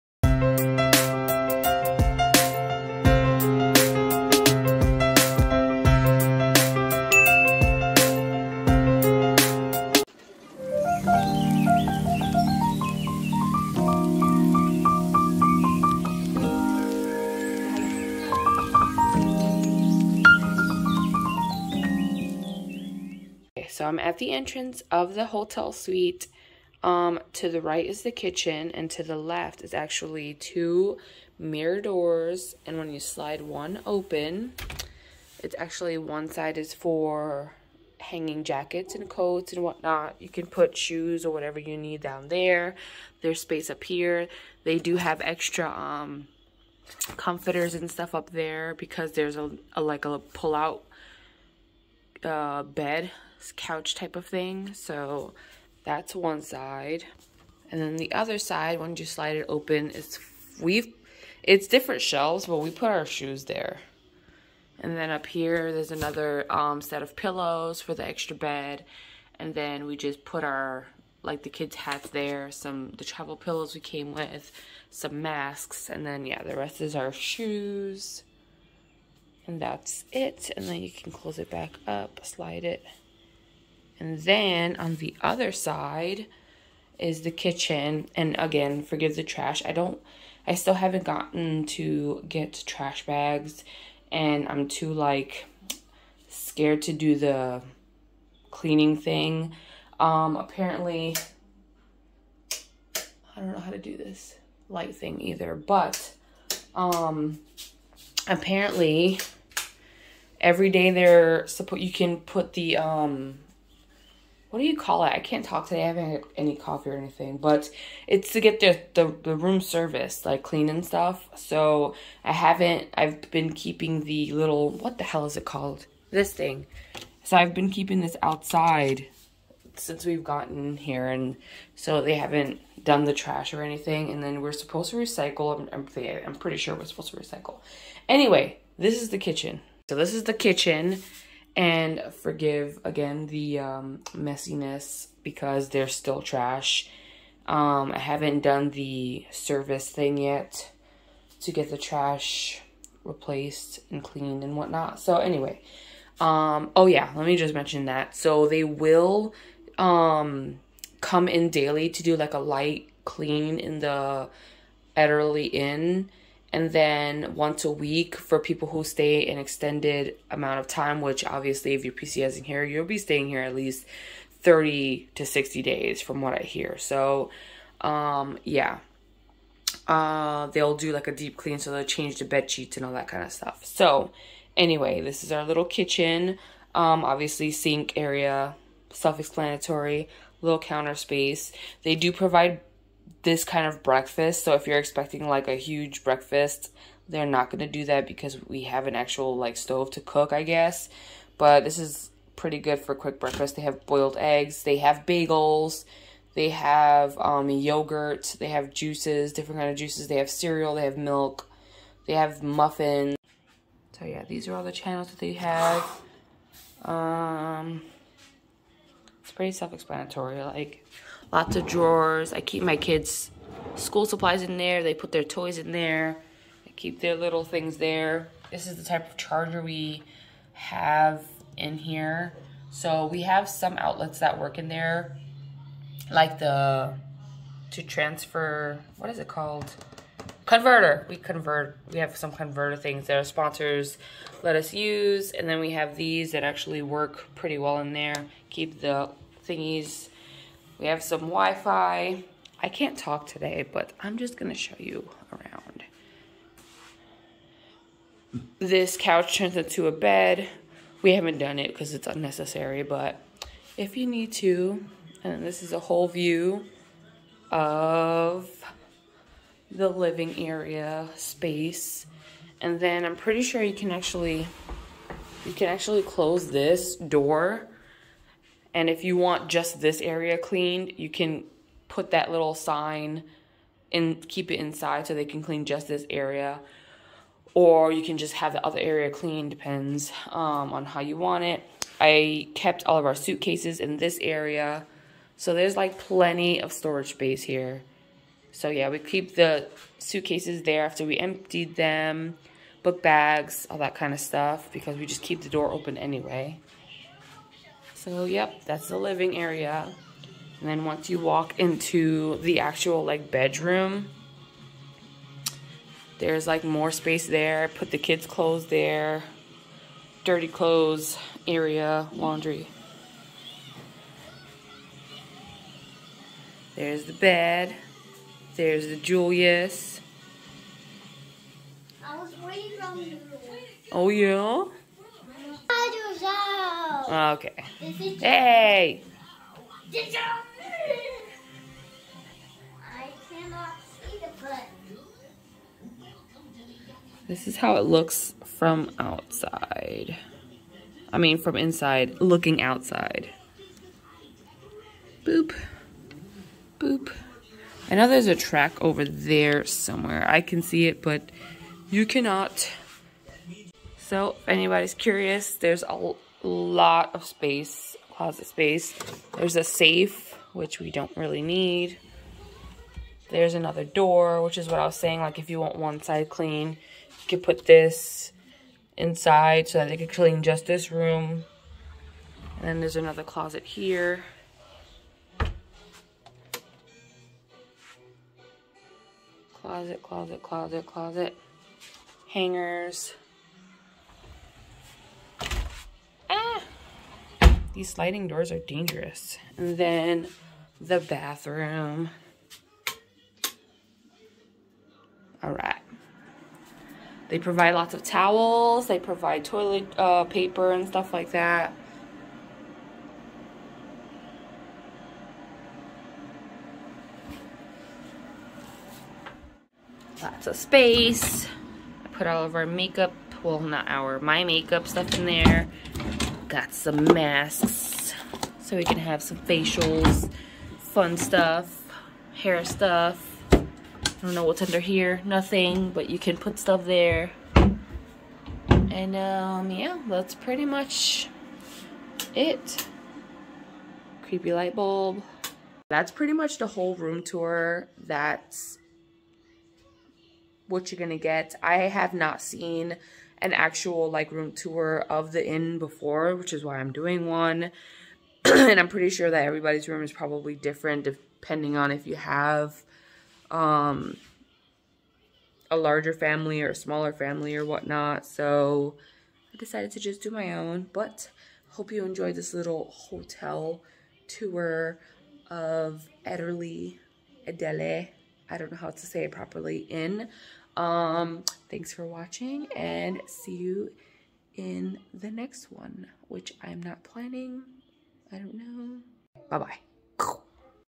Family Suite at Ederle Inn Vicenza, Italy
Room tour time of the family suite in Vicenza, Italy Ederle Inn